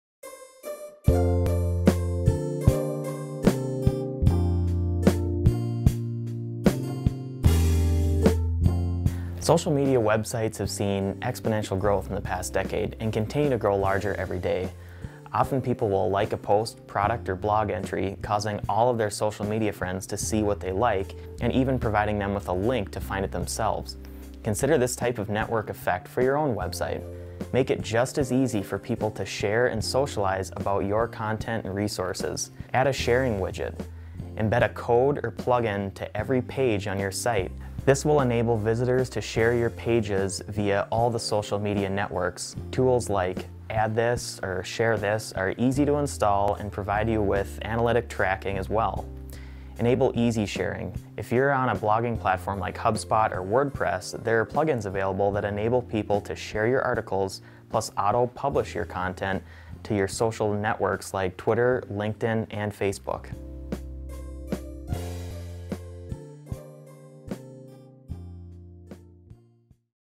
Website Must-Haves #18: Social & Shareable
Make it just as easy for people to share and socialize about your content and resources. Providing people with excellent content that can be shared with their peers will surely increase your flow of traffic.